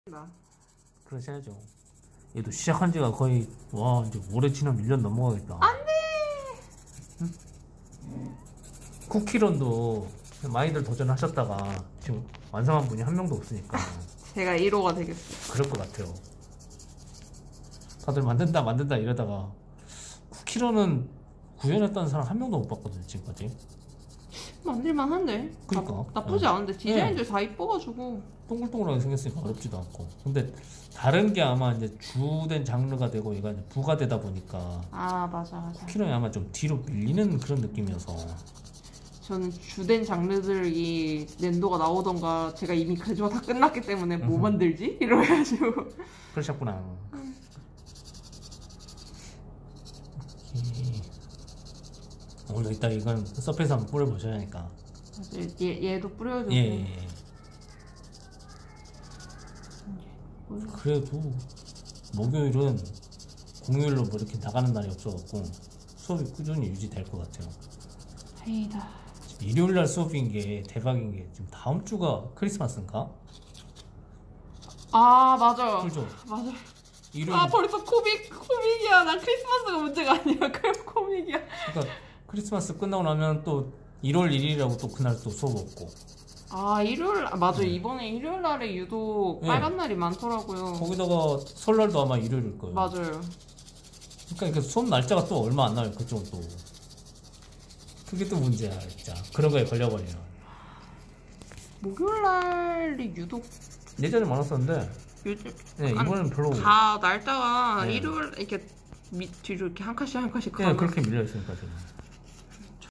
튜닝타임즈 강좌 롯데백화점문화센터 노원점 OOO님의 넨도 오비츠 커스텀 Cookie Run Charatcer 쿠키런 캐럭티 사포 연마 작업
롯데백화점문화센터 노원점 넨도로이드 오비츠 커스텀 수업 수강생 OOO님이 지난주에 서페이서를 뿌려서 표면 상태를 체크했던 머리 부품을 사포로 다듬어가고 있는데요, 얼굴 양쪽으로 귀엽게 돌아가는 머리카락에 에폭시퍼티를 덧붙여서 수정을 해줄까 고민하고 있습니다^^